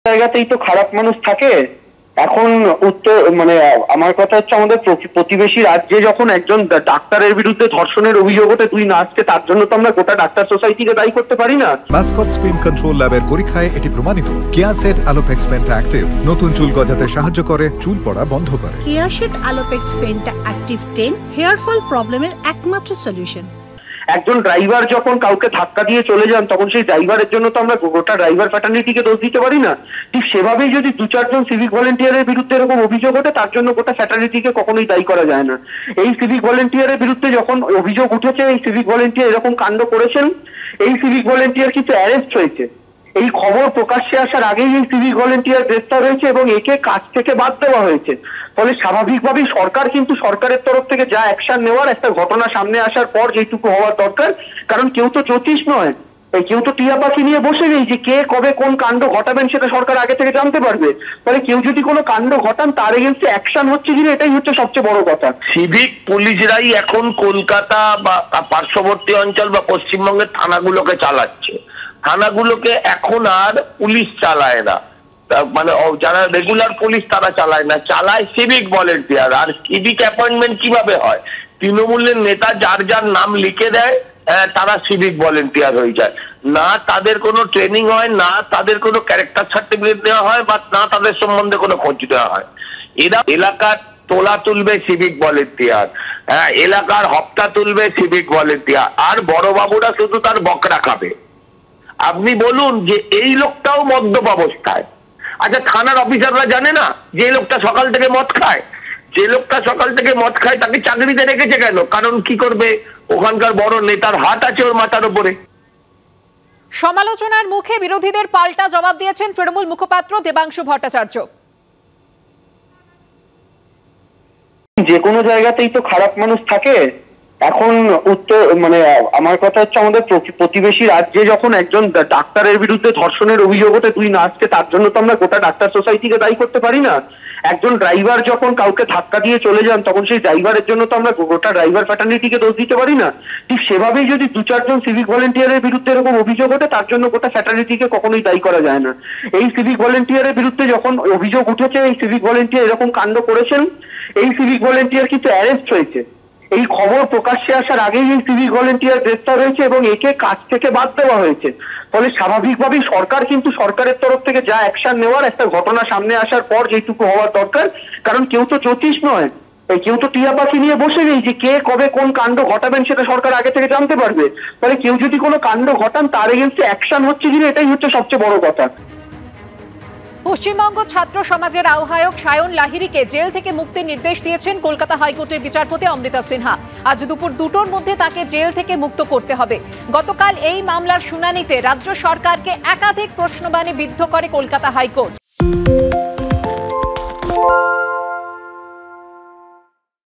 RG Kar News: সরকারের তরফ থেকে যা পদক্ষেপ নেওয়ার, তা নিয়েছে :দেবাংশু
ABP Ananda LIVE: 'এই খবর প্রকাশ্যে আসার আগেই সিভিক ভলান্টিয়ার গ্রেফতার হয়েছে এবং কাজ থেকে বাদ দেওয়া হয়েছে। ফলে স্বাভাবিকভাবে সরকার কিন্তু সরকারের তরফ থেকে যা অ্যাকশন নেওয়ার একটা ঘটনা সামনে আসার পর যেটুকু হওয়া দরকার তা করেছে', মন্তব্য দেবাংশু।